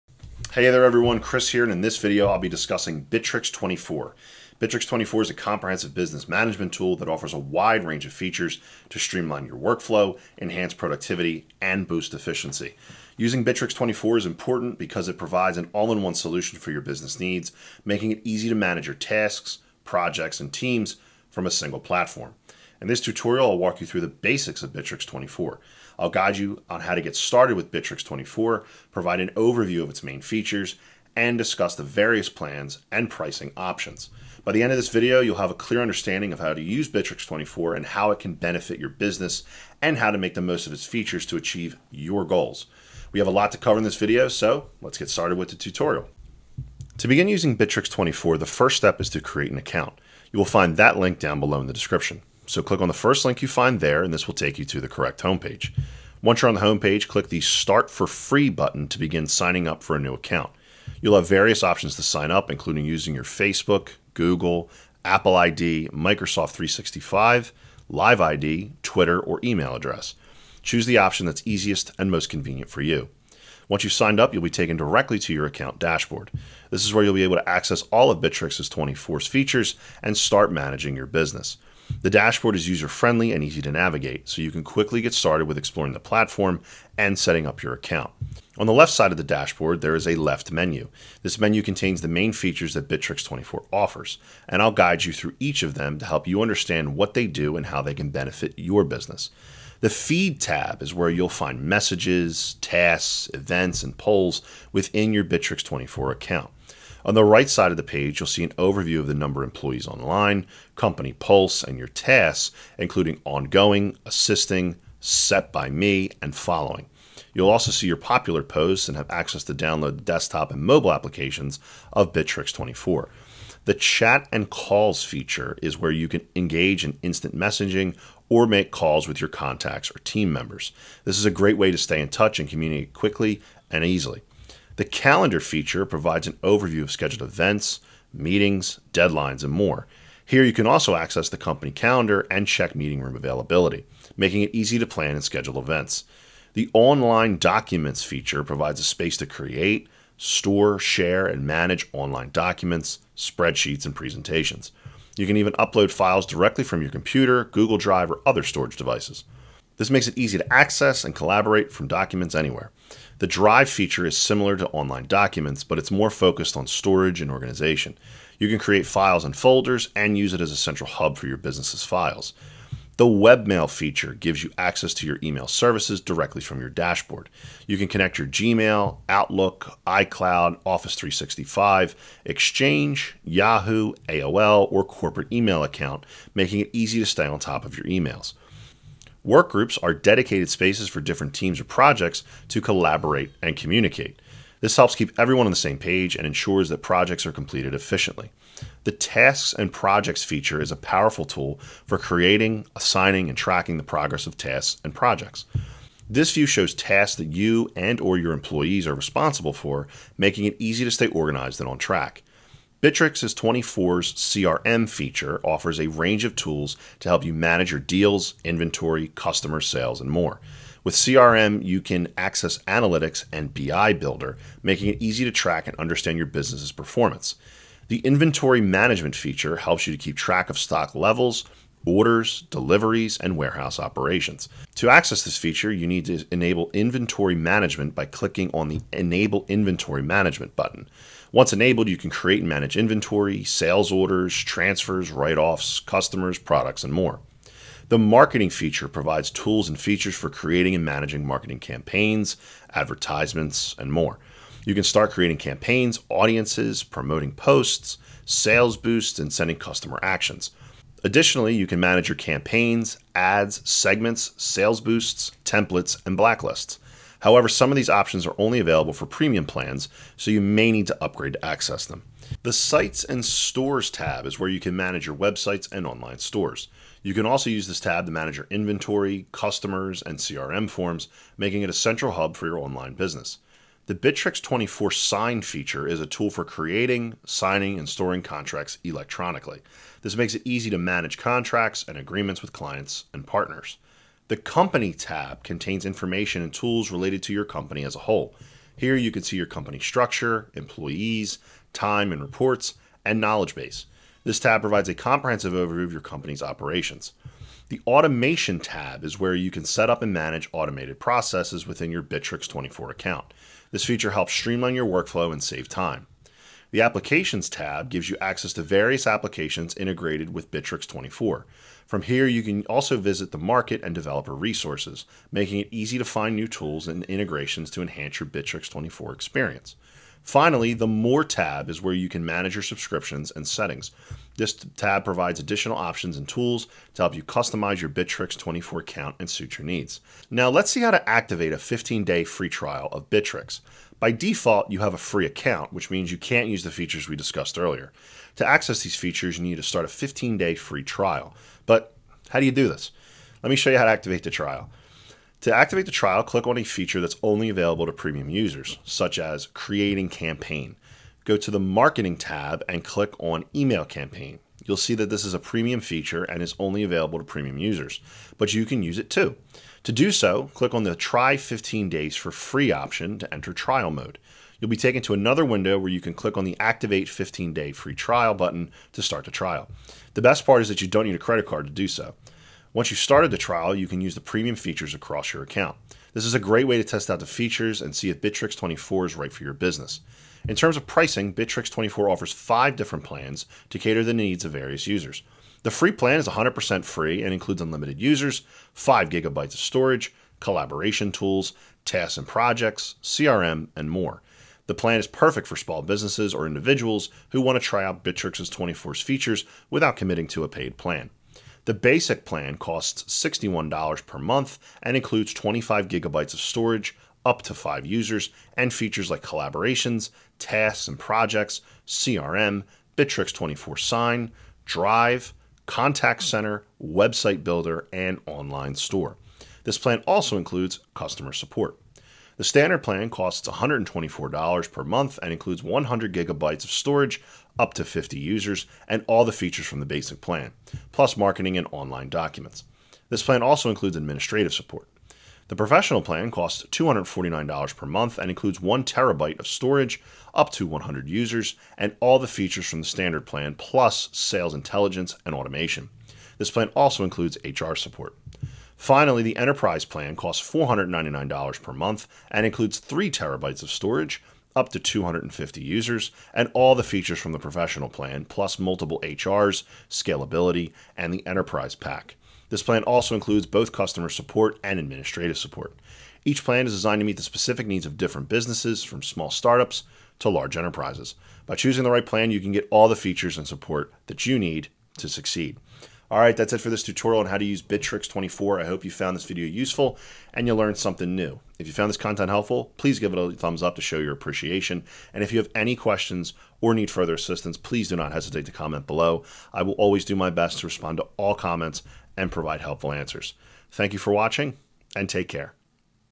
Bitrix24 Tutorial For Beginners | How To Use Bitrix24
This is my video tutorial on Bitrix24 for beginners. I cover the most basic functions.